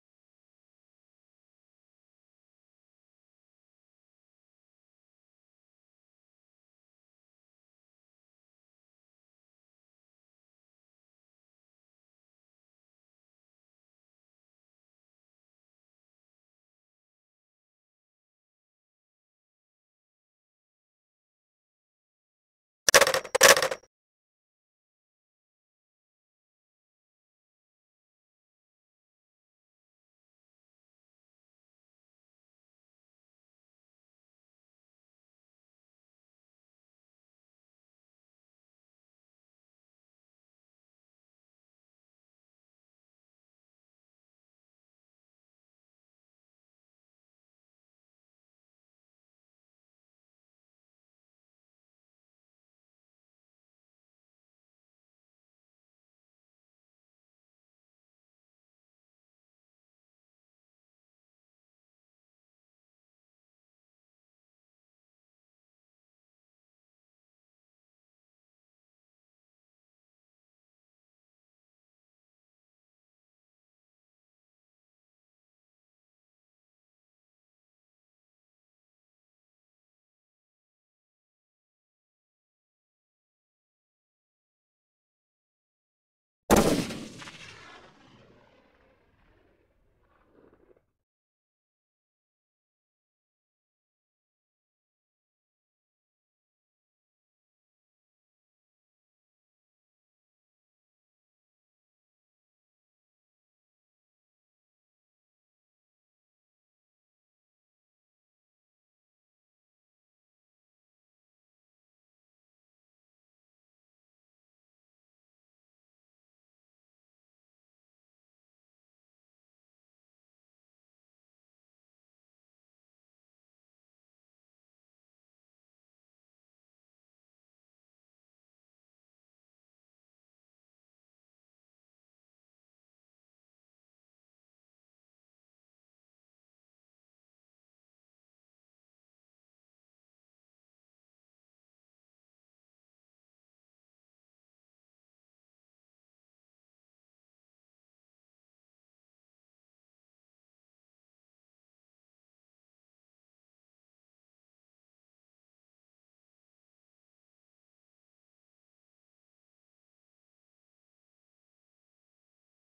Almal In Aksie Hoofstuk 5 (Sigbaar)
Hier is nuwe grappies van Almal in Aksie Komieks! Pause the video if you want to read.